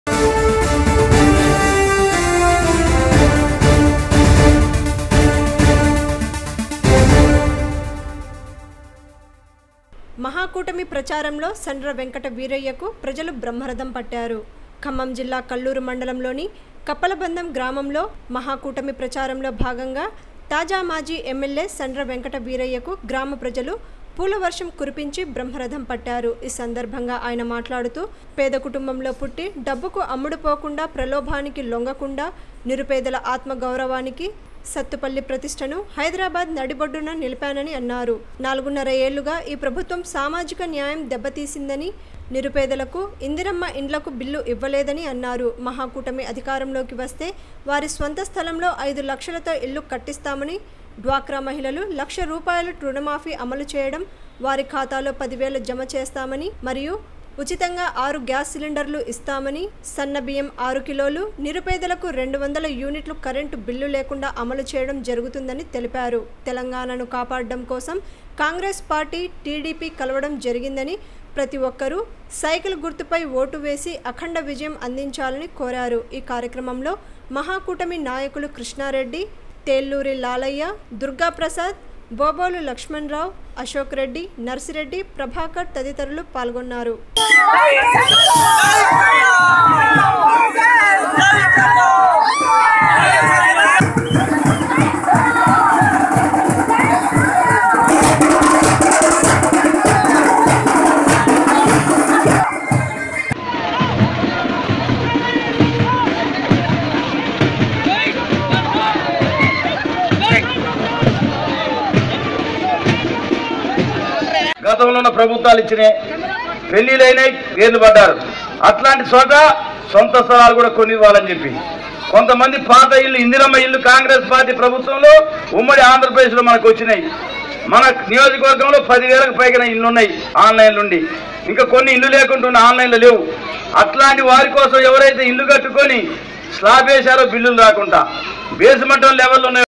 మహాకూటమి ప్రచారం లో సండ్ర కు బ్రహ్మరథం పట్టిన ప్రజలు || TS24 News
ఖమ్మం జిల్లా కల్లూరు మండలంలో ని కప్పలబంధం గ్రామంలో మహాకూటమి ప్రచారం లో భాగంగా తాజా మాజీ ఎమ్మెల్యే సండ్ర వెంకట వీరయ్య కు గ్రామ ప్రజలు పూలవర్షం కురిపించి బ్రమ్మరధం పట్టారు. *ఈ సందర్భంగా సండ్ర వెంకట వీరయ్య మాట్లాడుతూ పేద కుటుంబం లో పుట్టి డబ్బులు కు అమ్ముడు పోకుండా ప్రలోబానికి లొంగకుండా నిరుపేదల ఆత్మ గౌరవాన్ని ,సత్తుపల్లి ప్రతిష్ట ను హైదరాబాద్ నడి బొడ్డుపై నిలిపానని అన్నారు. నాలుగున్నర యేండ్లుగా ఈ ప్రభుత్వం సామాజిక న్యాయం దెబ్బతీసిందని,నిరుపేదలకు ఇంధరమ్మ ఇండ్లుకు బిల్లు ఇవ్వలేదని అన్నారు.మహాకూటమి అధికారంలోకి వస్తే వారి స్వంత స్థలంలో 5 లక్షలతో ఇల్లు కట్టిస్తామని, డ్వాక్రా మహిళలు లక్ష రూపాయలు రుణమాఫీ అమలు చేయడం, వారి ఖాతాలో 10 వేల జమచేస్తామని,ఉచితంగా గ్యాస్ సిలిండర్ 6ఇస్తామని, సన్నబియ్యం 6కిలోలు,నిరుపేదలకు 200 యూనిట్లు కరంట్ బిల్లు కట్టకుండా అమలు చేయడం జరుగుతుంది అన్నారు.తెలంగాణ ను కాపాడడం కోసం కాంగ్రెసు పార్టీ,టీడీపి కలవడం జరిగిందని ప్రతి ఒక్కరు సైకిల్ గుర్తు పై ఓటు వేసి అఖండ విజయం అందించాలని కోరారు. ఈ కార్యక్రమంలో మహాకూటమి నాయకులు కృష్ణారెడ్డి,తేళ్ళూరి లాలయ్య,చుక్కా, దుర్గాప్రసాద్, బొబోలు లక్ష్మణరావు, అశోకరెడ్డి,నర్సిరెడ్డి,ప్రభాకర్ తదితరులు పాల్గొన్నారు.